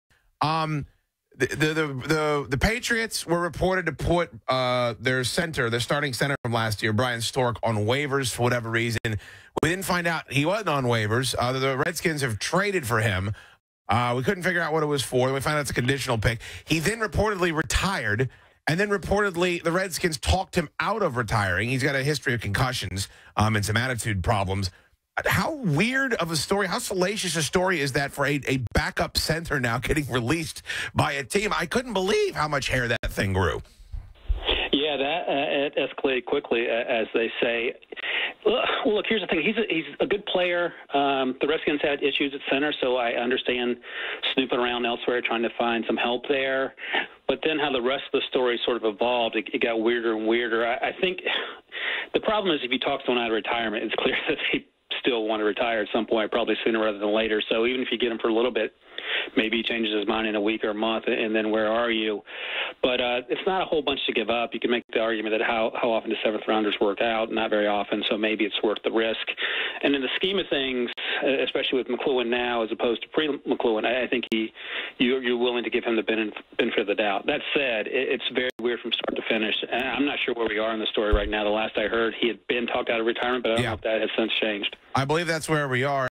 Report: Bryan Stork No Longer Mulling Retirement (Audio)
106.7 The Fan's Chad Dukes and Ryan Wilson of CBS Sports comment on the status of Bryan Stork. NFL Media’s Ian Rapoport reports Stork is no longer mulling retirement one day after being traded by the Patriots to the Redskins.